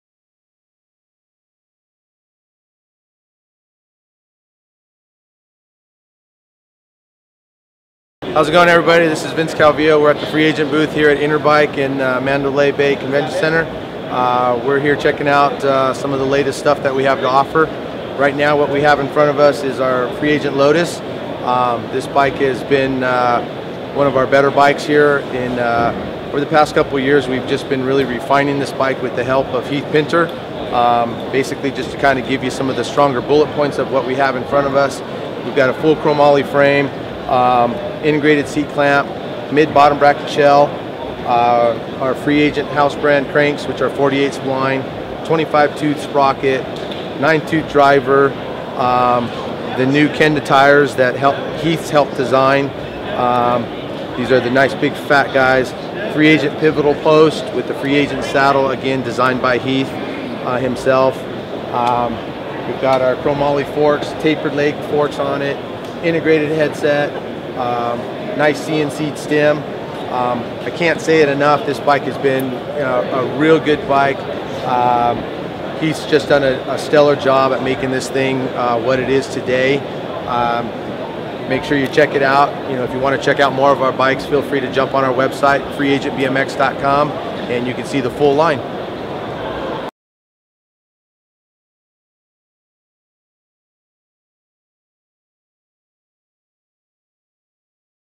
Free Agent Video Spotlight - Interbike 2013 - TransWorld RIDEbmx
Check out Free Agent's top of the line Lotus complete bike...